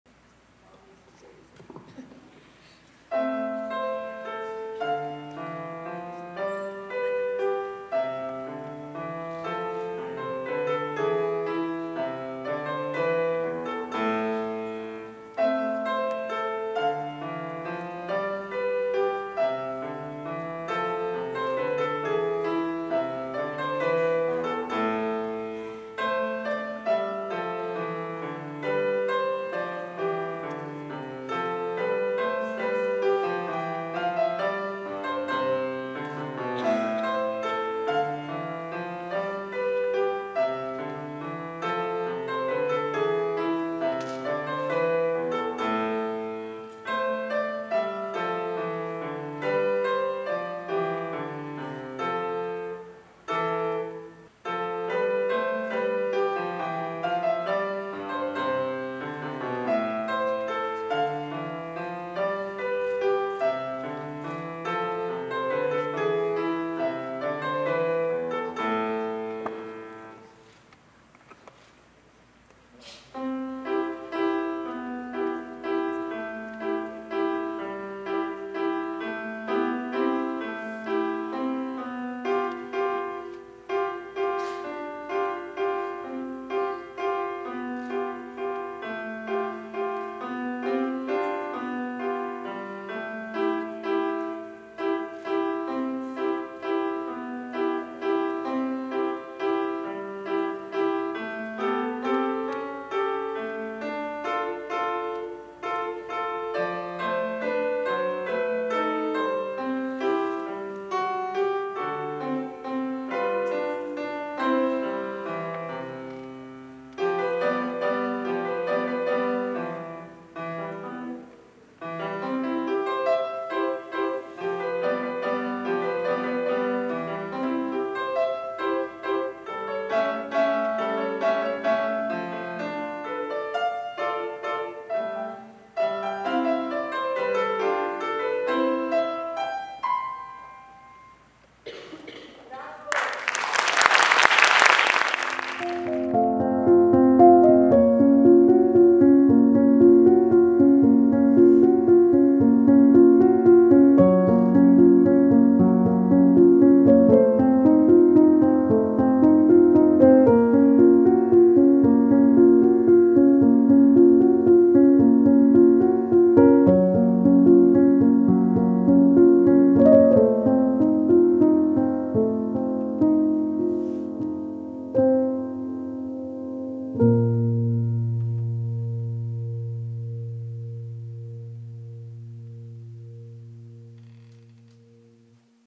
Músicas - Gao Gurgel e Johann Krieger
Apresentação da aluna LAÍS KIRSCHNER BRIZO DA SILVA, em um recital na Escola do Futuro de Goiás em Artes Basileu França.